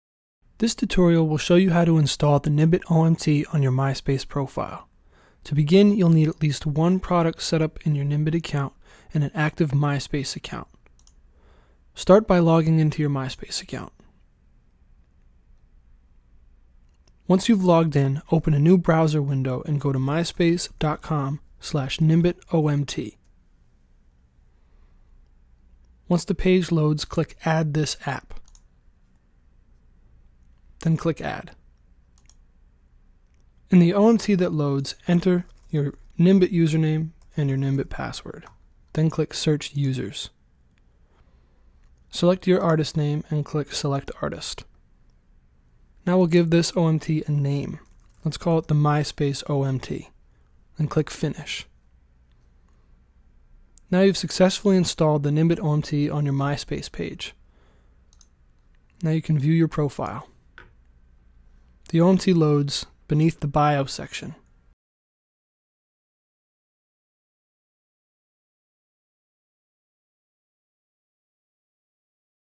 Sell Music on Myspace: Install the nimbitOMT Tutorial
Learn how to install the nimbitOMT onto your Myspace Profile and start selling music and merch on your Myspace!